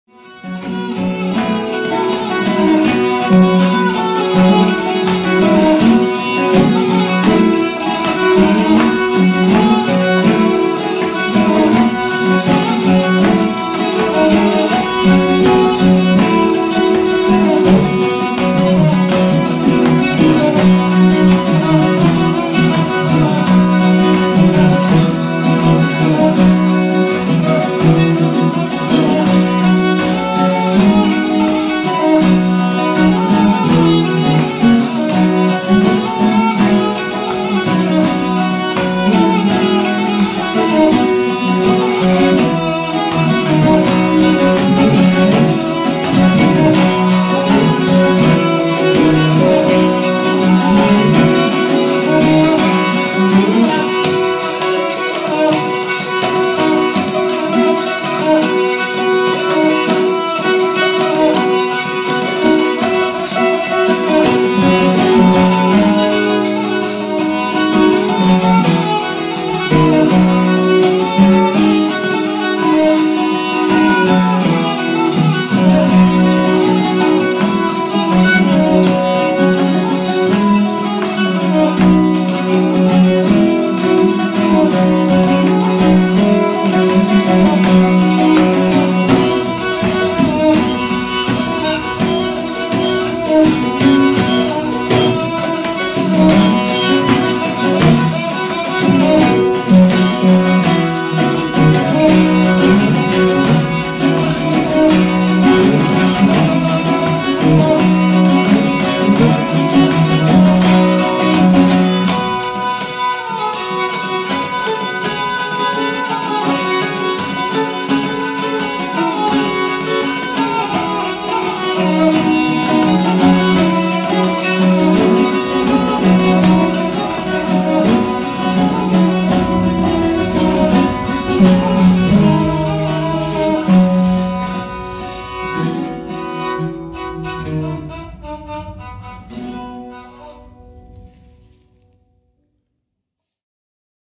Artur Abid & Anton Platonov khachesh music session
Khachesh-session with traditional Circassian musician Artur Abid
Takhtamukay, Adygea Republic, March 2016
Хачещ-сэйшн в Тахтамукае с Артуром Абид (шичепшин). Март 2016